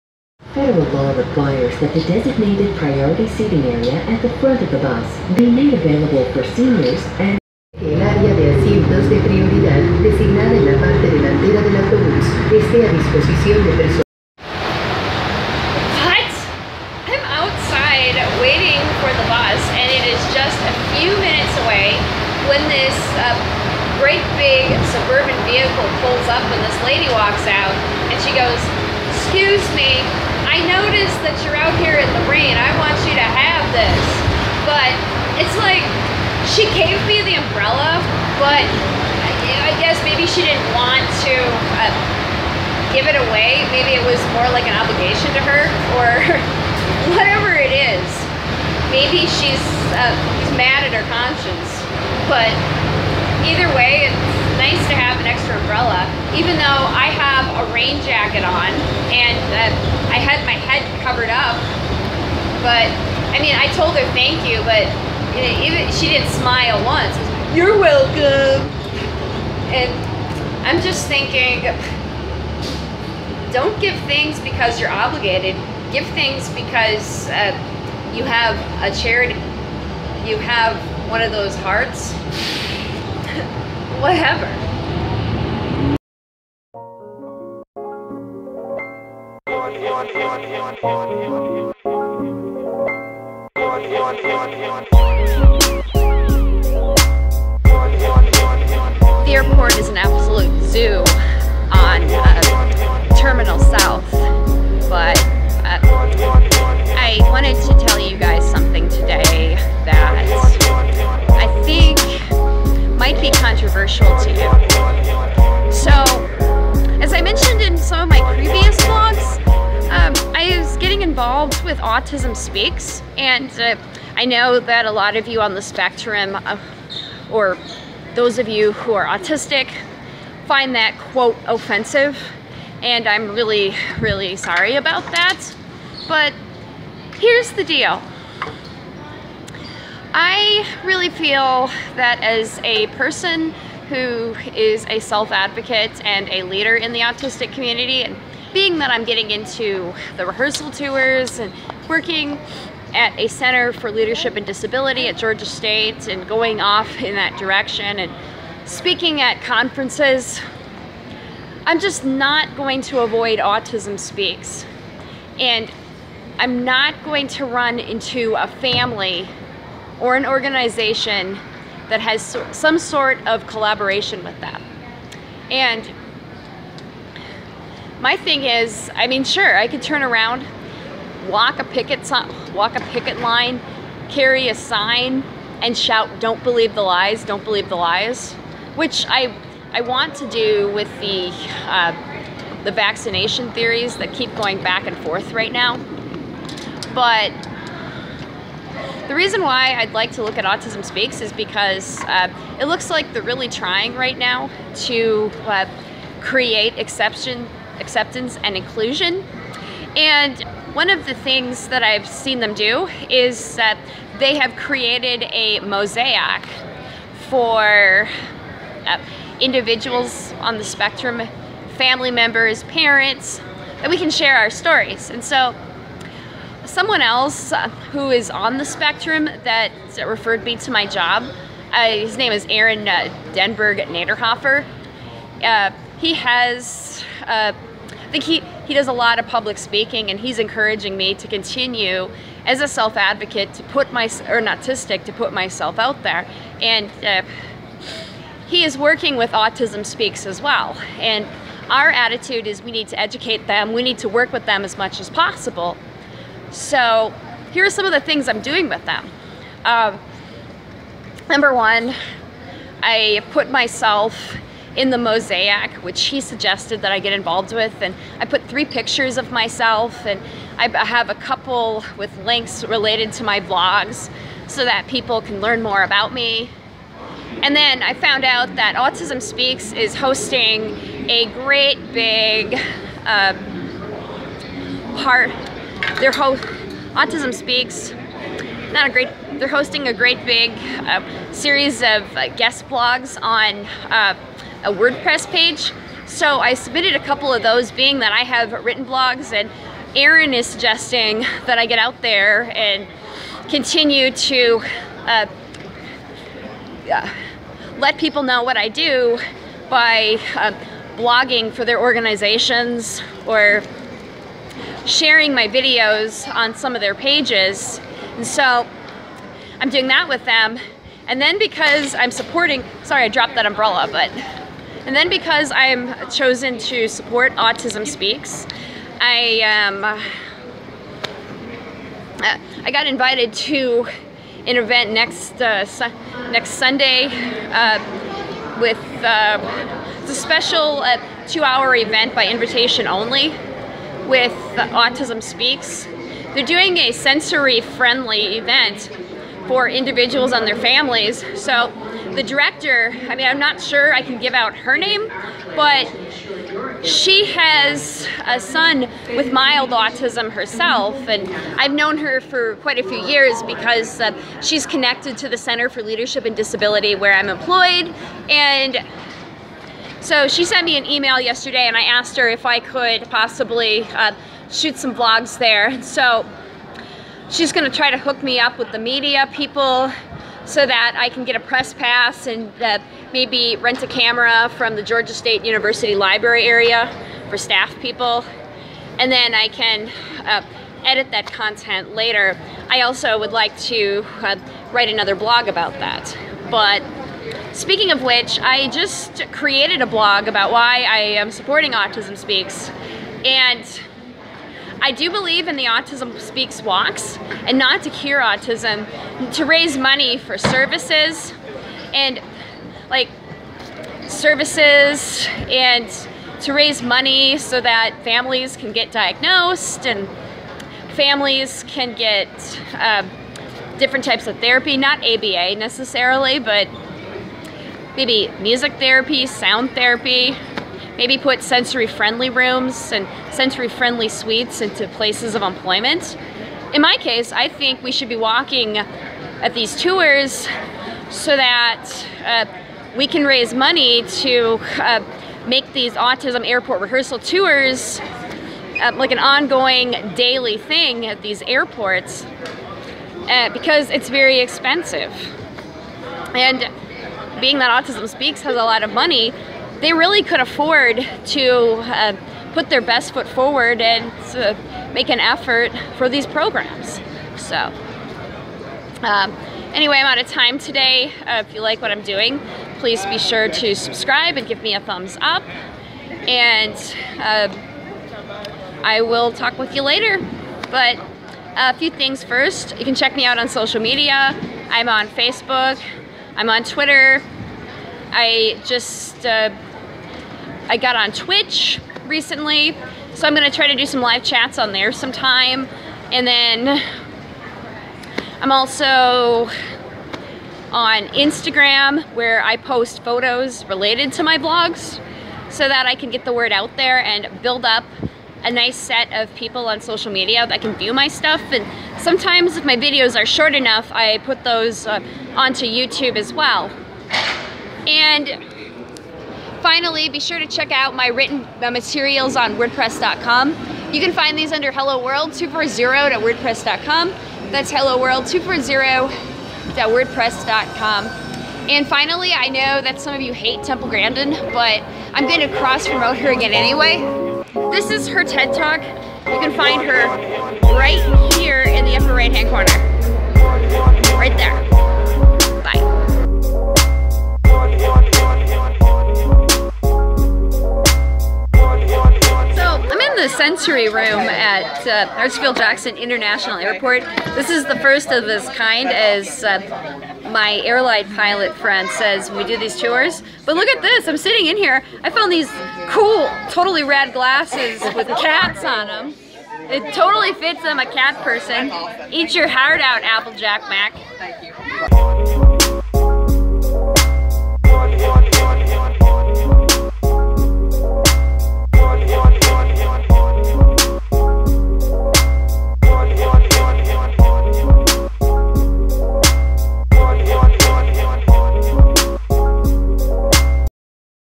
An Autistic who Supports Autism Speaks
Heaven forbid, I support Autism Speaks, the very organization that wanted to "Cure Autism" for many years while putting out theories on vaccination.   So why, as  an autistic would I support such an organization?  This video explains it.  If you want more information, check out this written blog on wordpress

Also, check out this mosaic that went live at the beginning of the month.